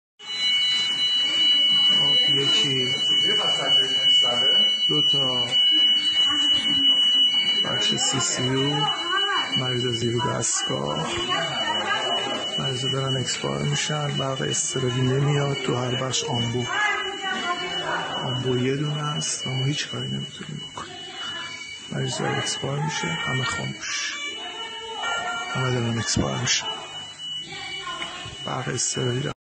دیشب قطع برق بخش ccu بیمارستان
۱۴تیر ۱۴۰۰
دیشب قطع برق بخش ccu بیمارستان که بیماران زیر دستگاه دچار وخامت حال میشوند

گاهی به دلیل کمبود وسایل مورد نیاز مدرن ٫برق اضطراری وصل نمیشود و حتی بیمار فوت میکند

چند نفر در این مدت که برقها قطع شده است فوت کرده اند !

جان مردم چقدر برای فرقه تبهکار ارزش دارد؟!!!